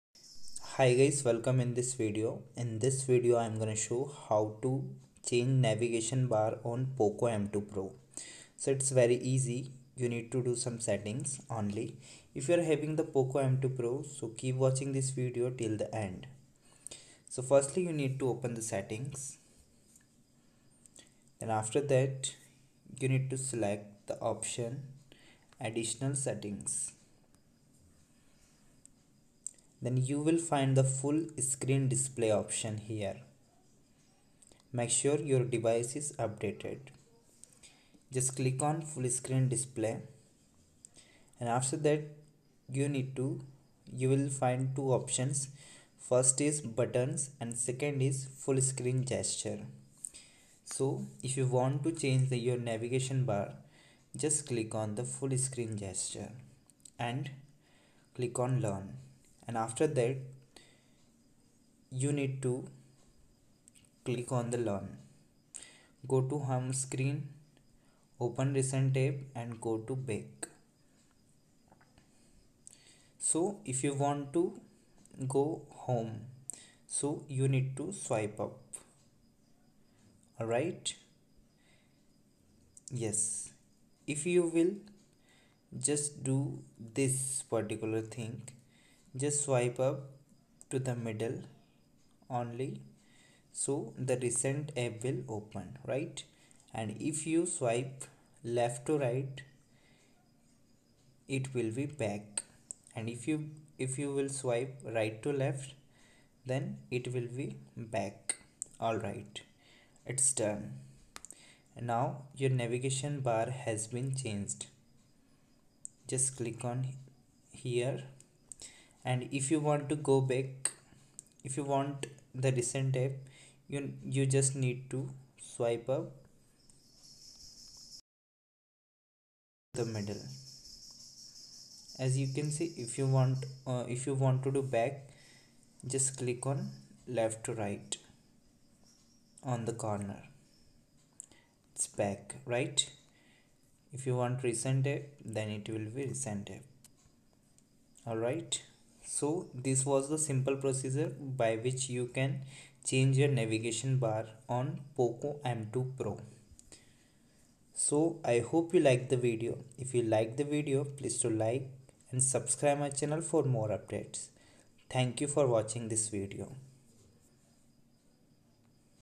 how to change navigation bar on poco m2 pro
How To Enable MIUI Gesture Navigation

To enable gesture navigation on MIUI, you should follow the steps mentioned below.

Firstly, head to the System Settings and choose Display

Now, you will see the 'Full-Screen Display' option. Tap on this option to enable gestures

Doing so, you will see the switch between apps, recent apps, and main screen gestures

The gesture controls provide more screen space as there is no need to have navigation buttons at the bottom of the screen. Also, users can swipe up from the navigation bar at the bottom to go to the home page. A right or left swipe from the edge of the screen will take you back, and an upward swipe will open recent apps. You can toggle between apps by swiping right or left on the navigation bar at the bottom.

Xiaomi MIUI 11, the custom ROM based on Android 10 launched last year comes with a slew of features such as smart reply, dark mode, improved privacy controls, and more. However, it misses out on one of the notable features seen on Android 10. Well, the talk is about Gesture Navigation and the company assured that it will rollout the same for its users.